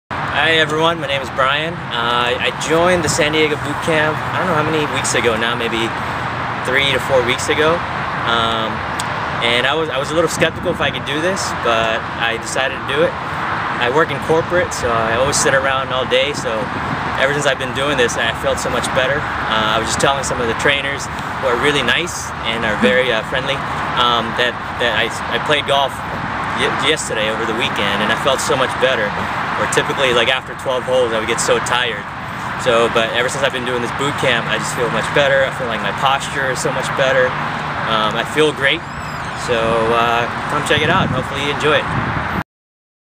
MAH09070.MP4
Bryan Calo shares his opinions on The San Diego Boot Camp, his golf game and new found strength!